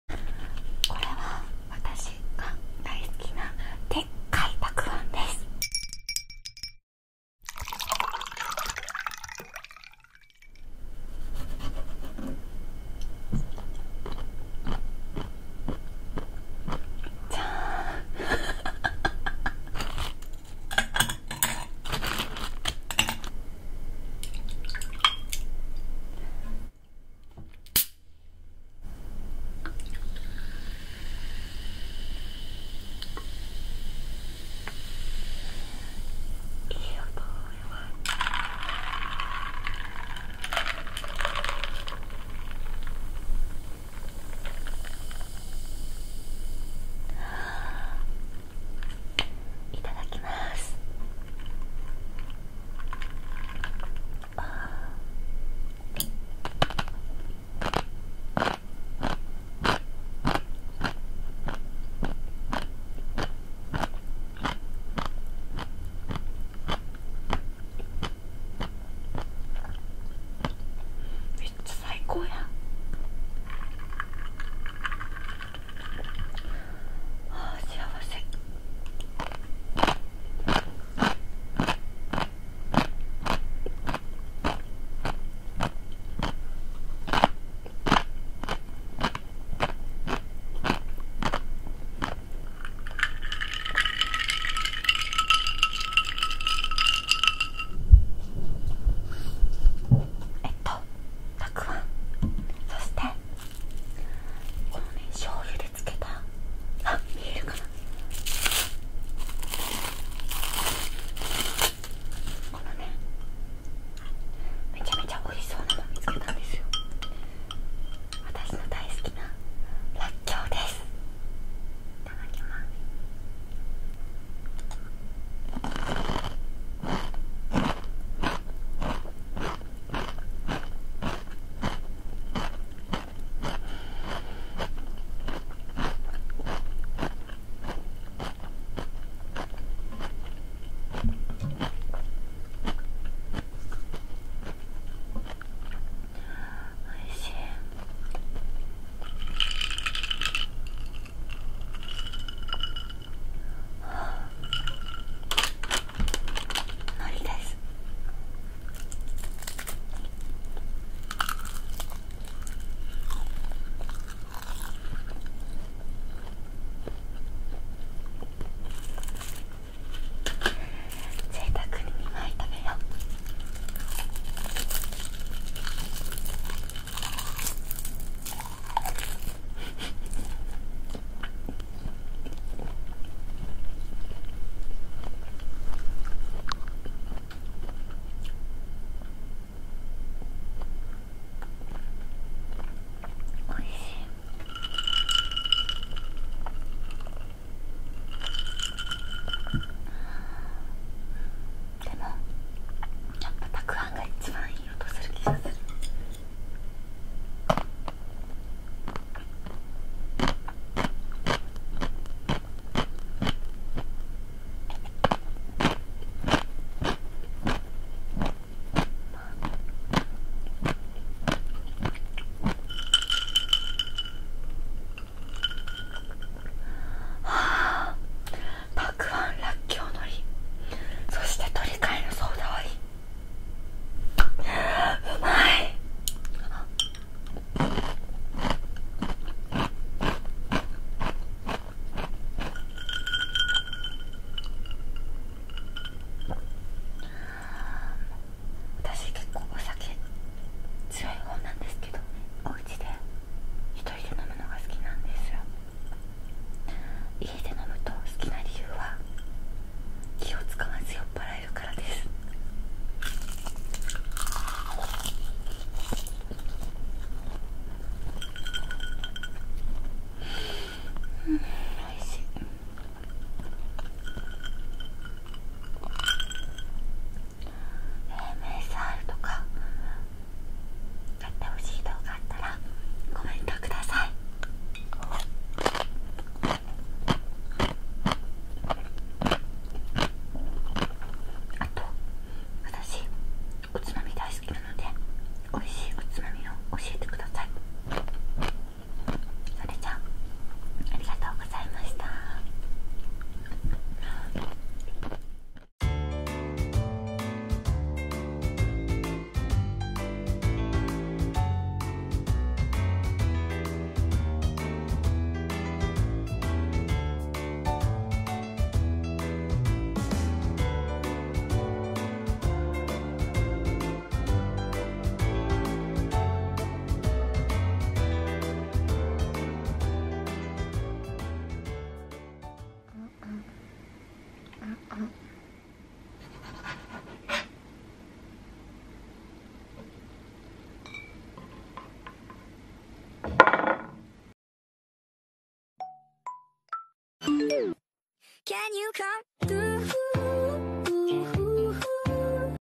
【ASMR】おうちで焼酎ハイボール飲みながら、たくあん食べてみた【炭酸音】【Eating Sounds】
前回スタジオで撮影してみたASMR、
今回はおうちで飲みながら第1弾！
わたしの好みがモロに出てるおつまみ達でお酒を堪能しました🍶
ぜひイヤホンで聞いてみてね！